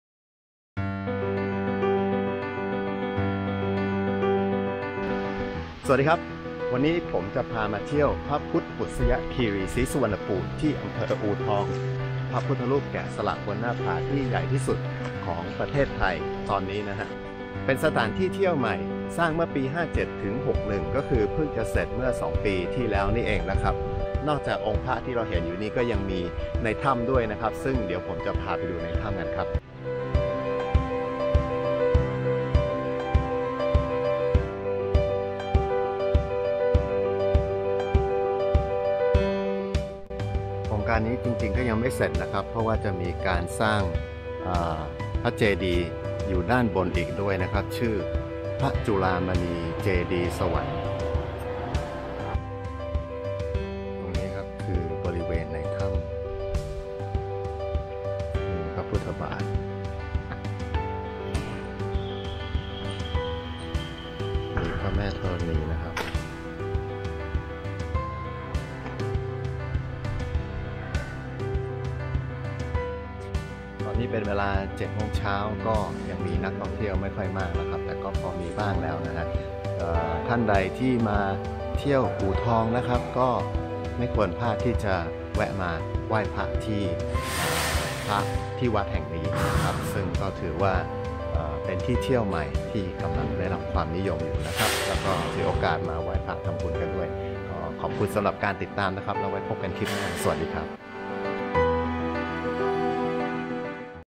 Let's Go to UTong ไปเที่ยวอู่ทอง ชมพระแกะสลักบนหน้าผาใหญ่ที่สุดกัน
อดเที่ยวนอก มาเที่ยวไทยกันดีกว่า Let's Go ขอเปลี่ยนแนวชั่วคราวมานำเสนอการเที่ยวในประเทศ รอบนี้คือจังหวัดสุพรรณบุรี อำเภออู่ทอง มีพระพุทธปุษยคีรีศรีสุวรรณภูมิ หรือหลวงพ่ออู่ทอง พระแกะสลักบนหน้าผา ใหญ่ที่สุดของประเทศไทย (ณ.ขณะนี้) ที่วัดเขาทำเทียม มาชมกันครับ